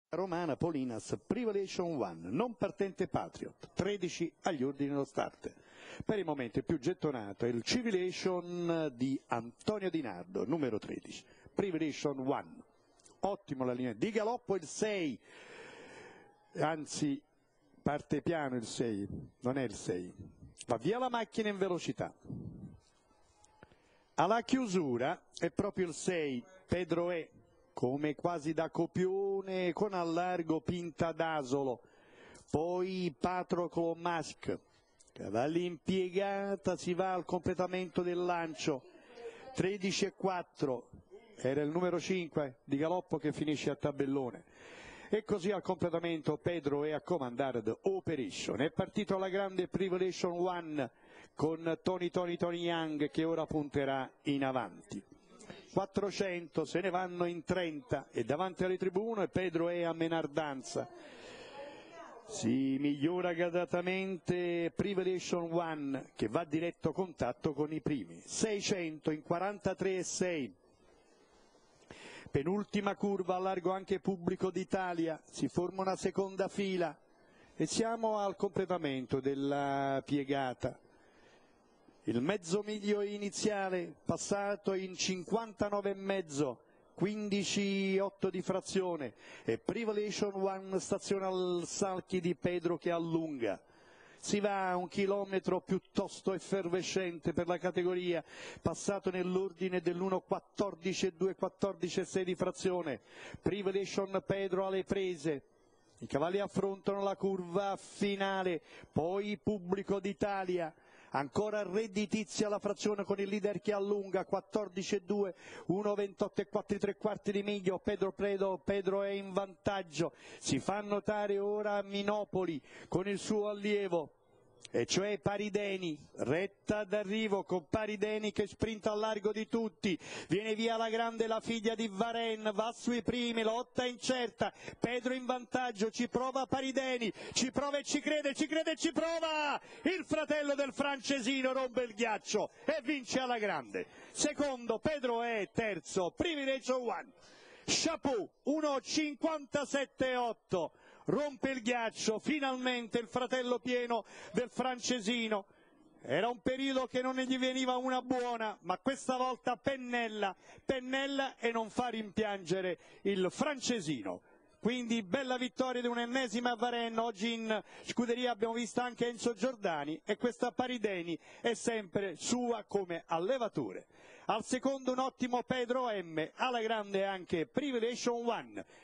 29/08/2013, Ss Cosma Damiano , Premio Ladrone Ok C.naz.
1. PARIS DANY (MINOPOLI V.), 2. PEDRO E' (DELL' ANNUNZIATA V P), 3. PRIVILACTION ONE (DI NARDO A.), 4. PATROCLO MASK (D'ALESSANDRO V. JR), 5. POLINASS (DI VINCENZO UMBERTO), 6. PETRA DIAMOND (MAISTO GIUSEPPE PIETRO), 7. PRINCELUIS VALLEY (LUONGO D.), 8. PUBBLICO D'ITALIA (LUONGO G.), 9. PINTA D'ASOLO (FORINO RAFFAELE), 10. PRANAYAMA (CIOTOLA GI.), 99. PRINCE GAMS (GALLUCCI R.), 99. PRUNUS (RICCIO GEN.), 99. PORTA ROMANA OK (DI NARDO GAETANO)